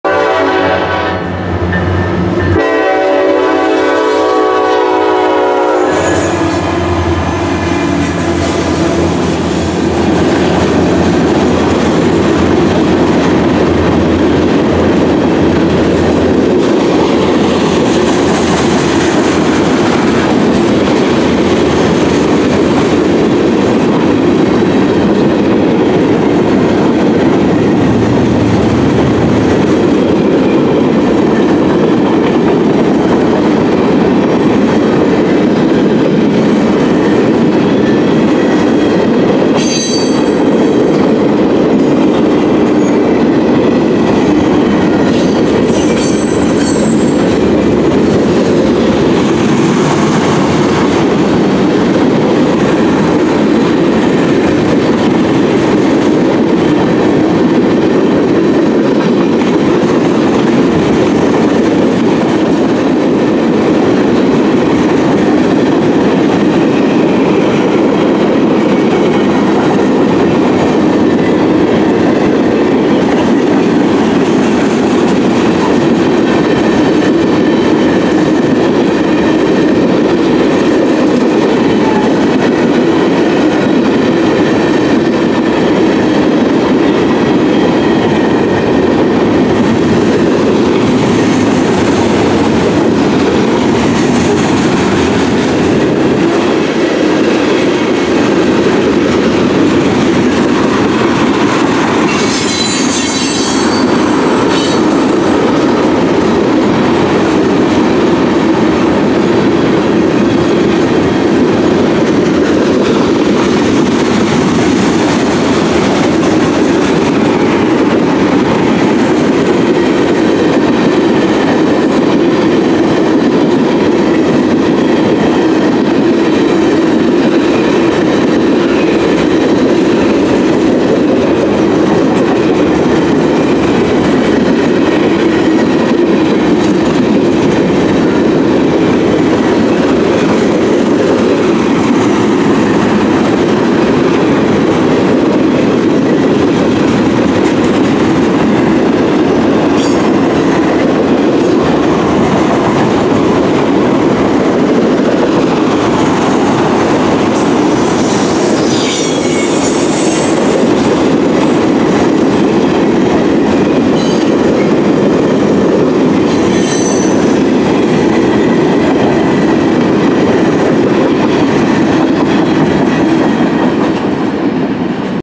09/16/23 - Rare CN SD70I trailing on CN M335-71 in Centralia, Illinois
The CN SD75I and SD70I are very similar units and are hard to distinct if you don't know your numbers. While CN has a bunch of SD75I units they only have 26 SD70I units (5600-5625) making them pretty rare to see. I was able to catch 5606 second out on M335 behind an SD70M-2. Enjoy!